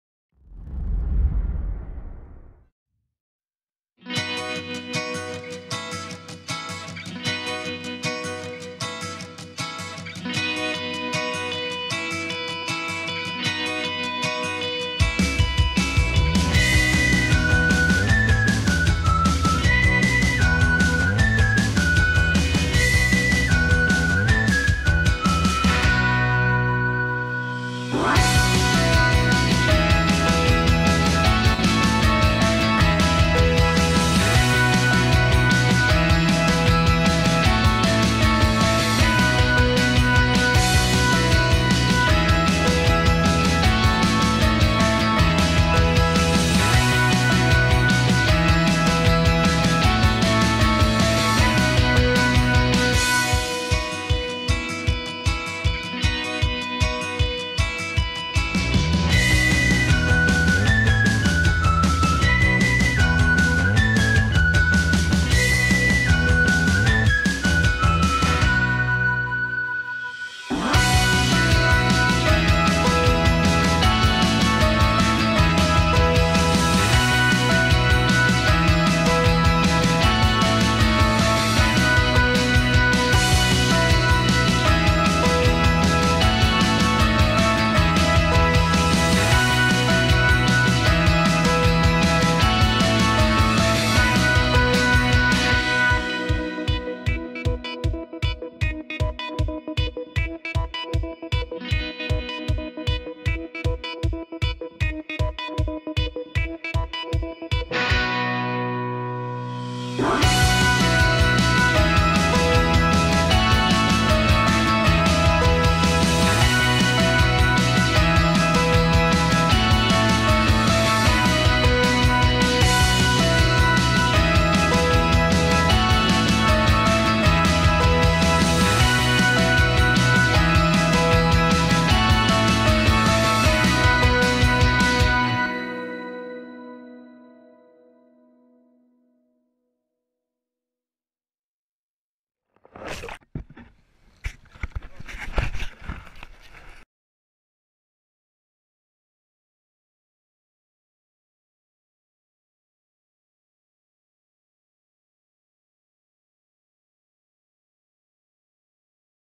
Nursery Function Research - Mngazana Estuary
A video of my BScHons research done with Rhodes University on the nursery role of the Mngazana estuary. The video was also entered in the Western Indian Ocean Marine Science Association (WIOMSA) video clip competition.